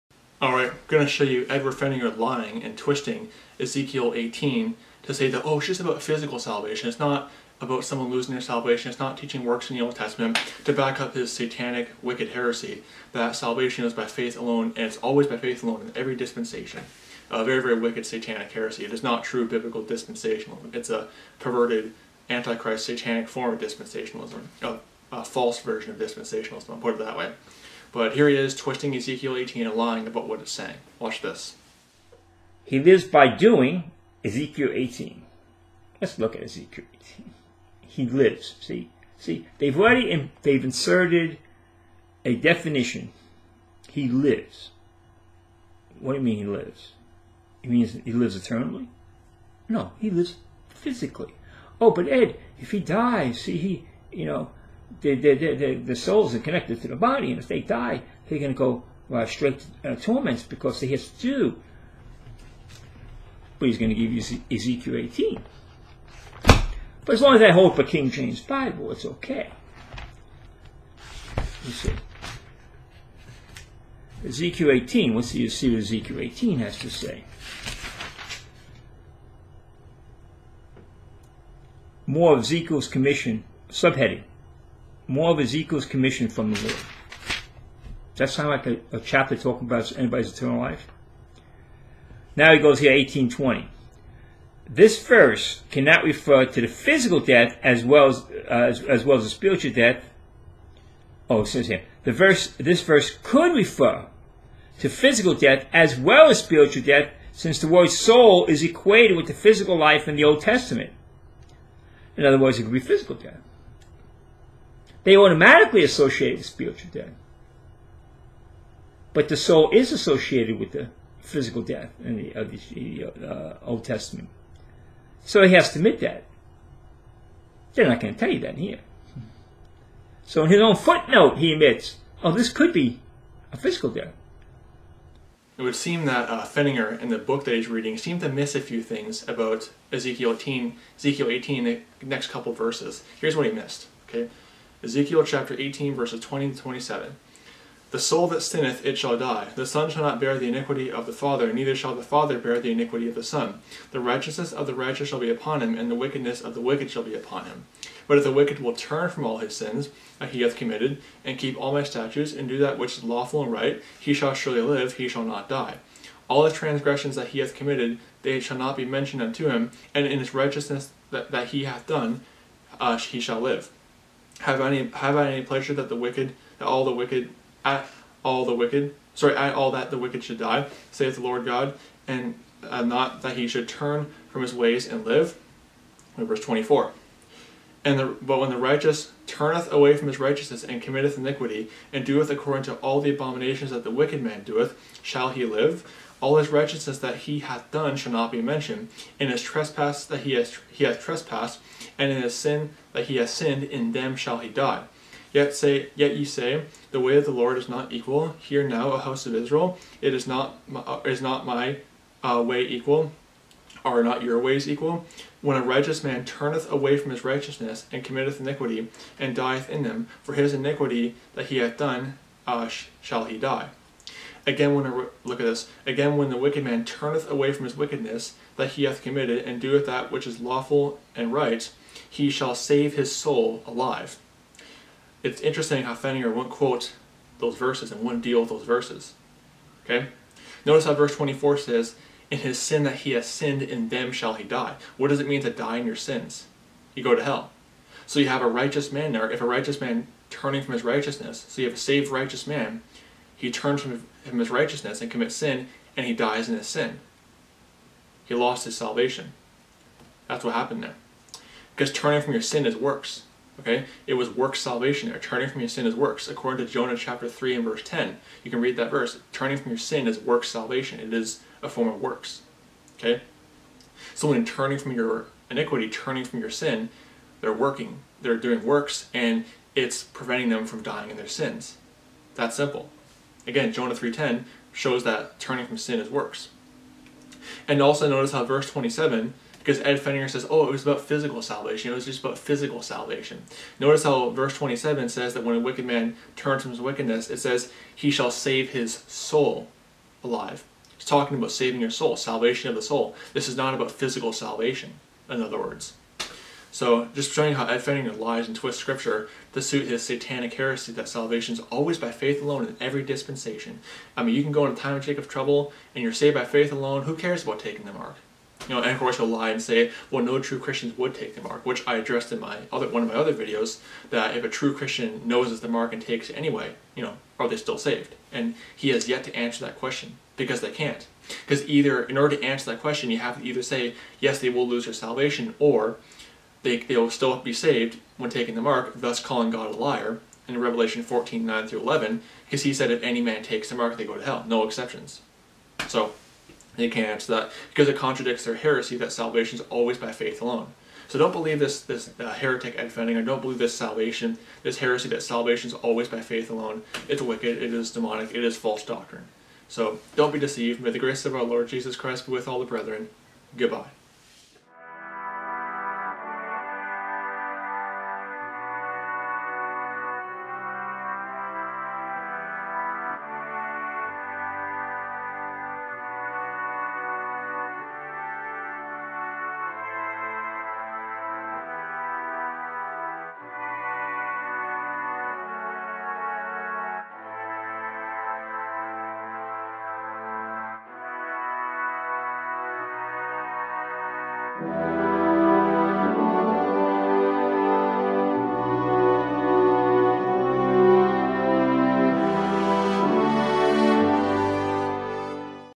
Ed Pfenninger Lies About Ezekiel 18
Ed Pfenninger lies and twists Ezekiel 18 to teach the exact opposite of what Ezekiel 18 is clearly teaching, in order to defend his wicked heresy that salvation was by faith alone in the old testament.